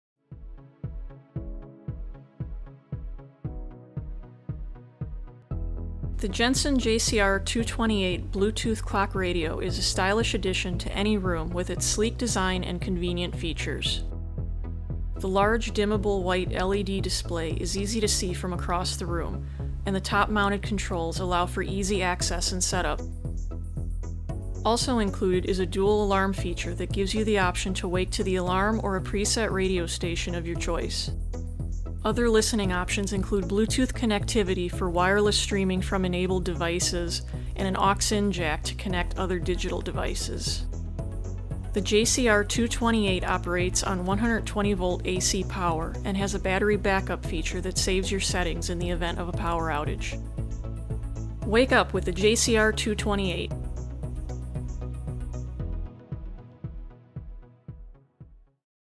Jensen JCR-228 Digital Bluetooth AM/FM Dual Alarm Clock Radio
The JENSEN JCR-228 digital Bluetooth dual alarm clock radio offers a stylish design and convenient features that can be used on a nightstand or around your home or office. The unit features Bluetooth connectivity for streaming music wirelessly from your Bluetooth-enabled smartphone or tablet. Two separate alarms let you wake to the alarm or radio, and the digital AM/FM receiver includes 10AM + 10FM presets for easy access. The 0.9" white digital LED display is easy to see from across the room and compliments the overall design. Other features include a hi/low dimmer switch, sleep function and volume control. The unit operates on 120V AC power, and has a battery back-up feature for saving your time settings in case of a power outage.

- Supports A2DP, AVRCP
- AM/FM Digital PLL Receiver
- 30AM + 30FM Preset Stations
- 0.9” White Digital LED Display
- Auxiliary Input Jack for Connecting Your iPod, MP3 or other Digital Audio Players
- Dual Alarm Clock with Snooze Function
- Wake to Radio or Alarm
- Sleep
- Volume Control
- Hi/Low Dimmer
- Power: AC 120V/60Hz
- Battery Back-up for Clock: 2 x ‘AA’ (UM-3) (Not Included)